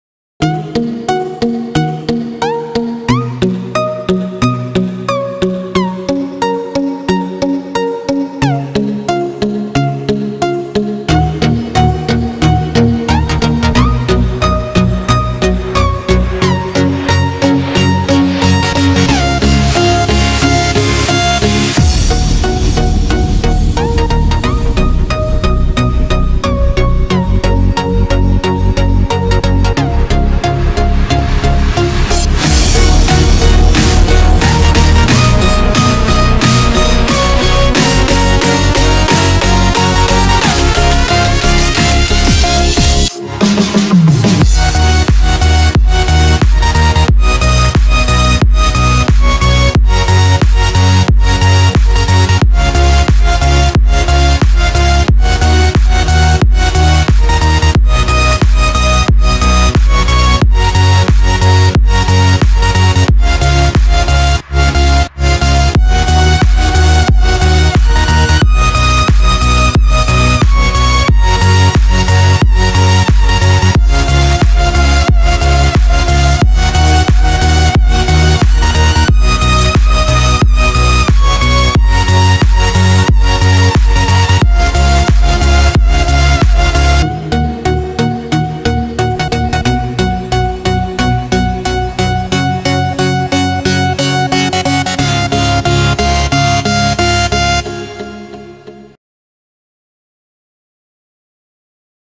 večka kula, starigrad paklenica
Ruins on the beach in Starigrad, Paklenica - Croatia, Velebit mountains in the back.
Taken with DJI Mavic Air drone in 4k. Watch in highest quality.